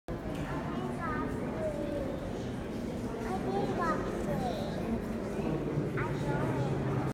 Klimt's Alley in front of Castle Chamber at the Legion of Honor
Klimt's landscapes at the Legion of Honor's Klimt & Rodin exhibit blew me away.